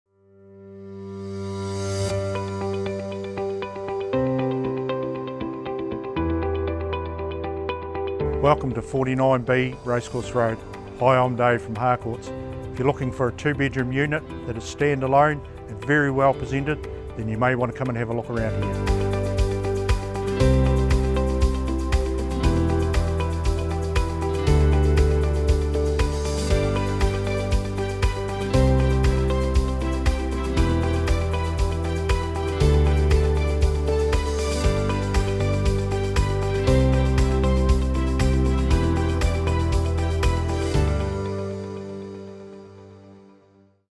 FOR SALE - 49B Racecourse Road, Glengarry - Harcourts Invercargill
Stand Alone
If you are looking to downsize then this property may be worth considering. Low maintenance and well presented. Two generous sized bedrooms, north open plan living/dining with floor mounted heat pump, up-to-date modern kitchen with good appliances. Good bathroom with separate shower and bath. Internal access garage, garden shed, and easy-care gardens. Close to many amenities.

Consultants:
Dave Hegarty